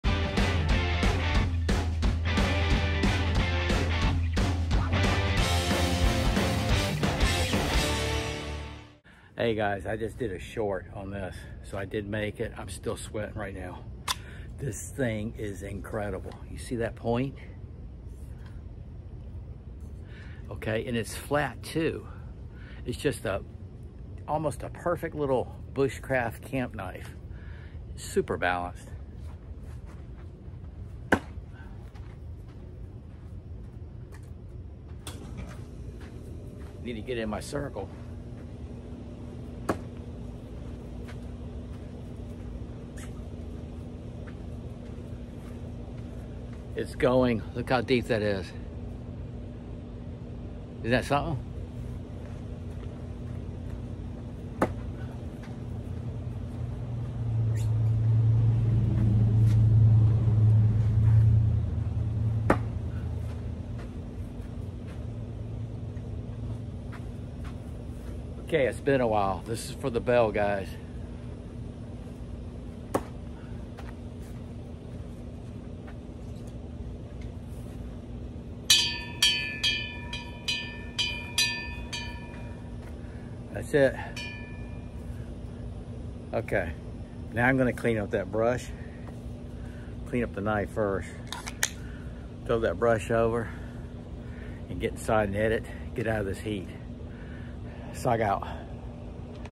QSP Canary Fixed Blade Knife from the Shop to throwing in the Board
Perfect weight and balance